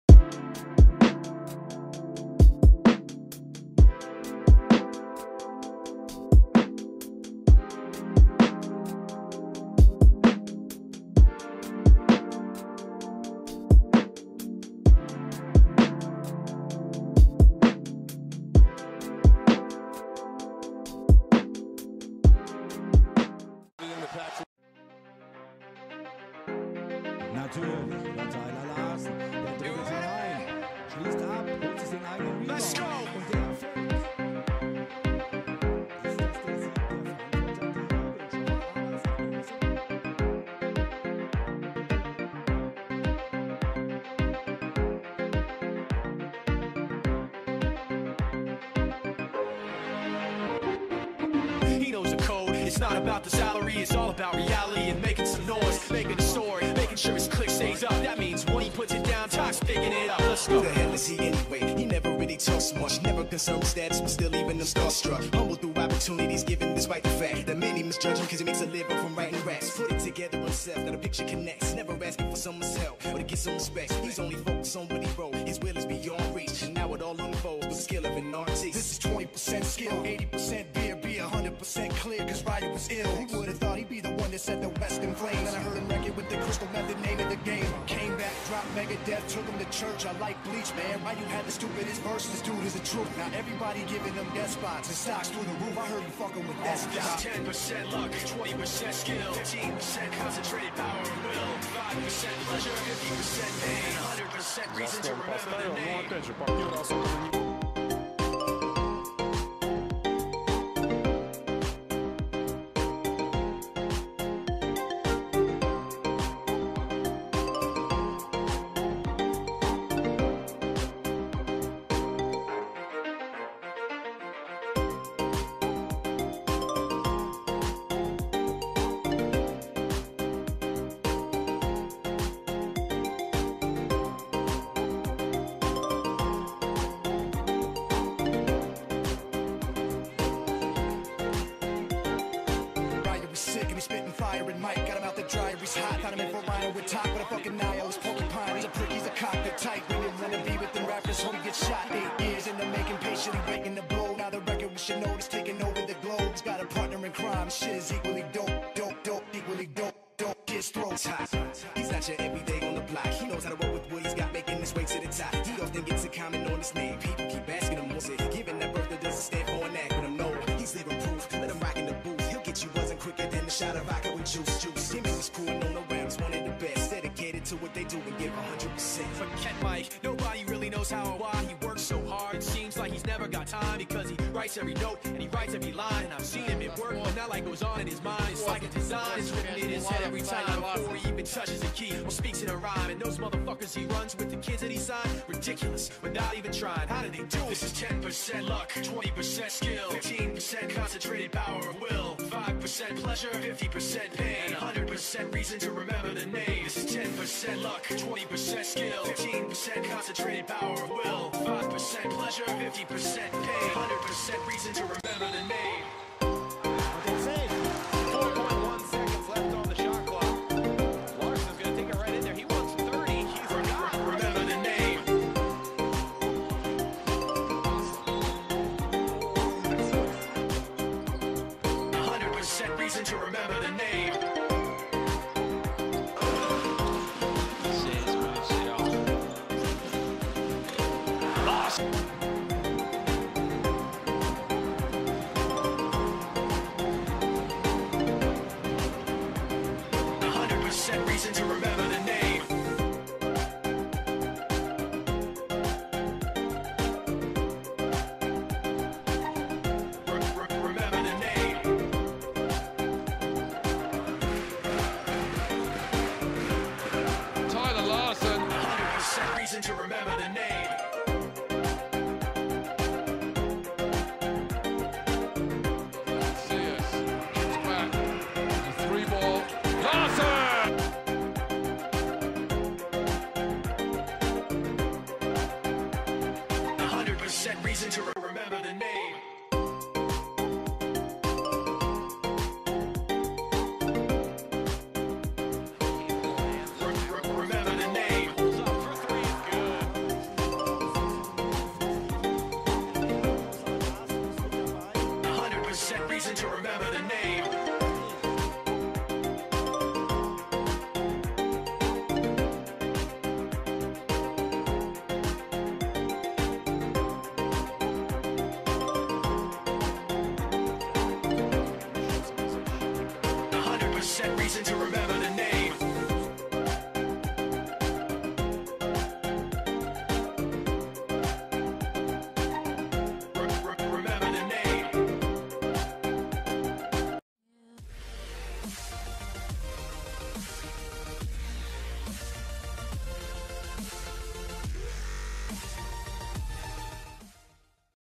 Tyler Larson ● Career Best Plays & Highlights ● 2015-2020
Tyler Larson G  #55

Current Team: EWE Baskets Oldenburg

Born: Dec 31, 1991 (28 years old)

Birthplace/Hometown: Las Vegas, Nevada

Nationality: United States

Height: 6-3 (191cm)     Weight: 185 (84kg)

support us!